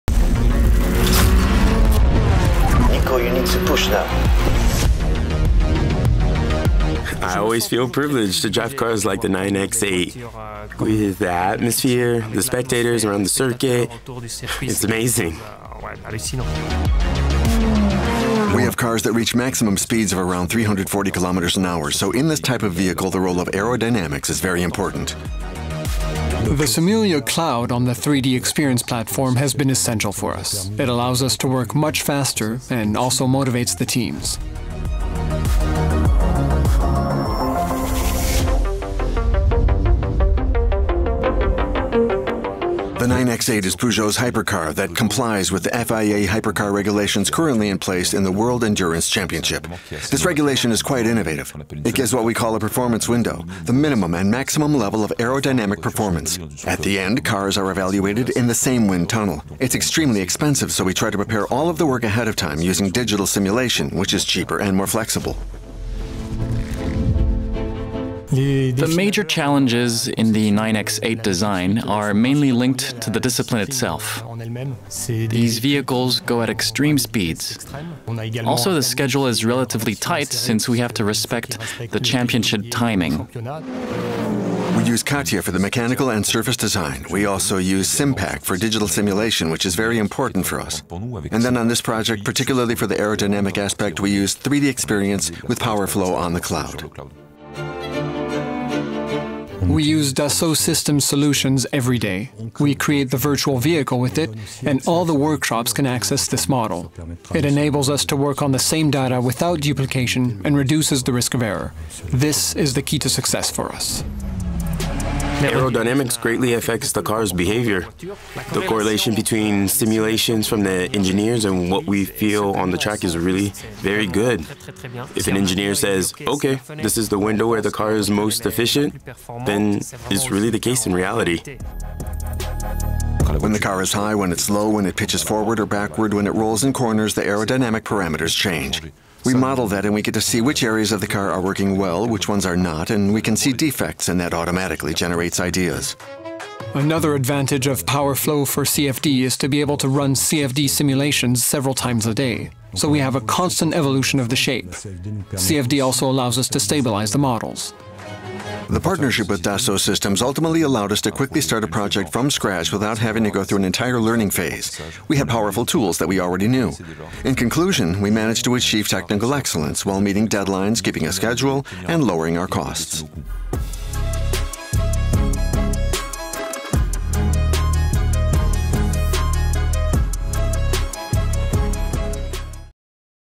Peugeot Sport - Accelerating performance with dynamic simulation ǀ Dassault Systèmes
Building the new PEUGEOT 9X8 Hybrid Hypercar from scratch, Peugeot Sport partnered with Dassault Systèmes and adopted the 3DEXPERIENCE platform to gain aerodynamic performance while saving significant time, money and resources in the process.

I always feel privileged
to drive cars like the 9X8

with the atmosphere, the spectators
around the circuit, it's amazing.

We have cars that reach maximum speeds
of around 340 km/h.

So in this type of vehicle,
the role of aerodynamics is very important.

The SIMULIA cloud on the 3DEXPERIENCE platform
has been essential for us.

It allows us to work much faster
and also motivates the teams.

The 9X8 is PEUGEOT's Hypercar that complies
with the FIA Hypercar regulations

currently in place in the World Endurance Championship.
This regulation is quite innovative.

It gives what we call a performance window -
the minimum and maximum level of aerodynamic performance.

At the end, cars are evaluated in the same wind tunnel.
It's extremely expensive,

so we try to prepare all of the work ahead of time
using digital simulation which is cheaper and more flexible.

The major challenges in the 9X8 design
are mainly linked to the discipline itself.

These vehicles go at extreme speeds.

Also, the schedule is relatively tight
since we have to respect the championship timing.

We use CATIA for the mechanical and surface design.

We also use [SIMULIA] Simpack for digital simulation,
which is very important for us.

And then, on this project, particularly for the aerodynamic aspect,
we use 3DEXPERIENCE with [SIMULIA] PowerFLOW on the cloud.

We use Dassault Systèmes solutions every day.

We create the virtual vehicle with it,
and all the workshops can access this model.

It enables us to work on the same data
without duplication, and reduces the risk of error.

This is the key to success for us.

Aerodynamics greatly affects the car's behavior.

The correlation between simulations from the engineers
and what we feel on the track is really very good.

If an engineer says: "OK, this is the window where the car
is most efficient," then it's really the case in reality.

When the car is high, when it's low, when it pitches forward or backward,
when it rolls in corners, the aerodynamic parameters change.

We model that and we get to see which areas
of the car are working well, which ones are not,

and we can see defects,
and that automatically generates ideas.

Another advantage of [SIMULIA] PowerFLOW for CFD
is to be able to run CFD simulations several times a day.

So we have a constant evolution of the shape.
CFD also allows us to stabilize the models.

The partnership with Dassault Systèmes ultimately allowed us
to quickly start a project from scratch

without having to go through an entire learning phase.
We had powerful tools that we already knew.

In conclusion, we managed to achieve technical excellence
while meeting deadlines, keeping a schedule, and lowering our costs.